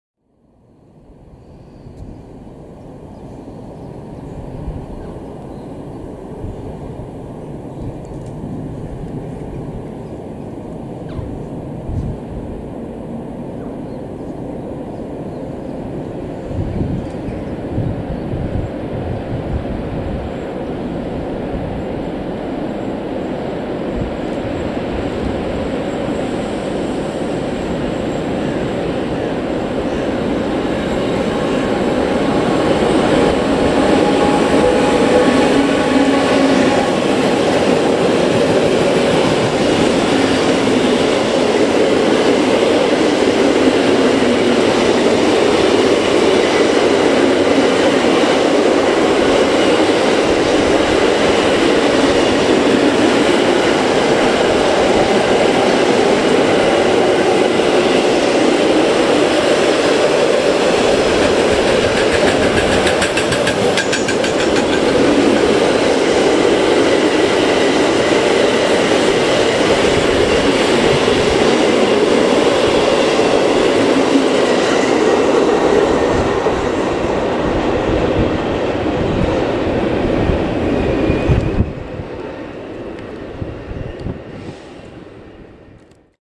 Captrain 1619 met Gefco-autotrein door station Amsterdam Bijlmer ArenA!
De fraaie in Raillogix-kleuren bestickerde 1619 van Captrain komt met een volbeladen Gefco-autotrein onder de karakteristieke stationskap van station Amsterdam Bijlmer ArenA.